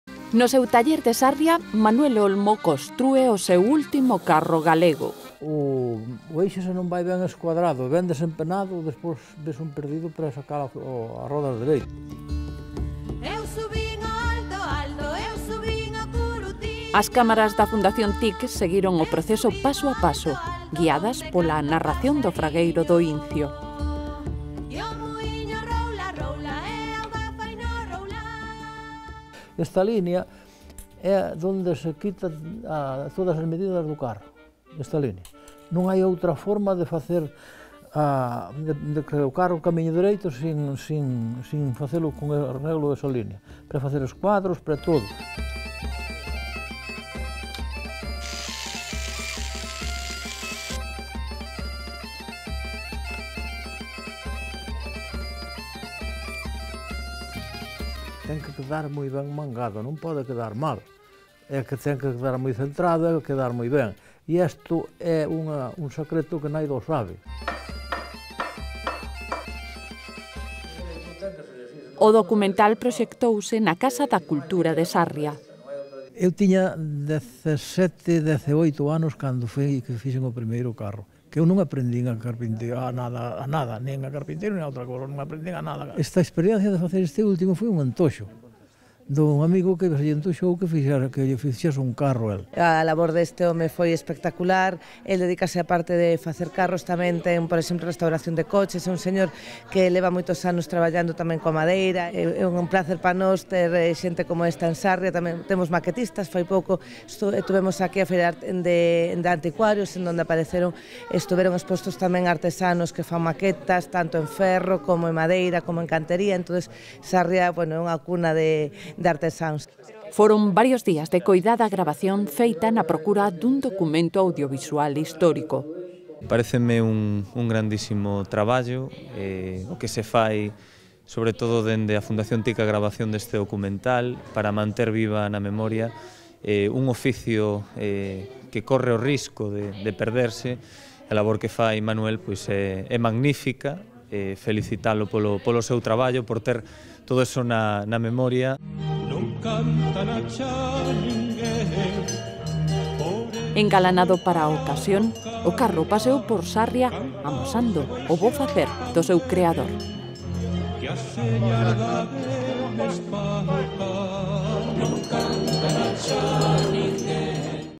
A Fundacion TIC presenta un documental sobre a construción do carro tradicional
No seu taller Sarria, Manuel Olmo costrúe o seu último carro galego.
As cámaras da Fundación TIC de Lugo seguiron o proceso paso a paso guiadas pola narración do fragueiro do Incio.